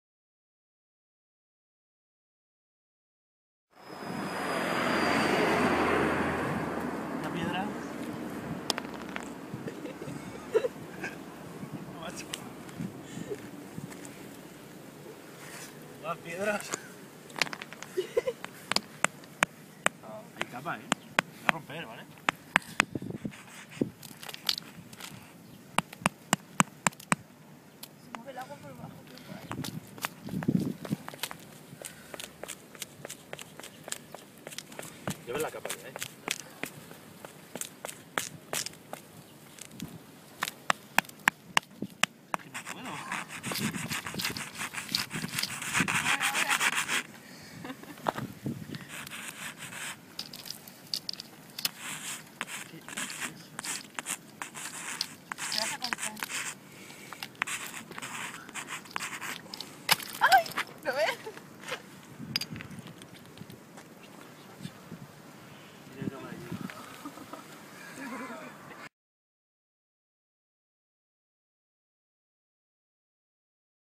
Helada en Torrevieja (4/Febrero/2012) (2)
Helada en el Alto de la Casilla en Torrevieja (Alicante) en la mañana del 4 de Febrero de 2012. Proyecto Mastral.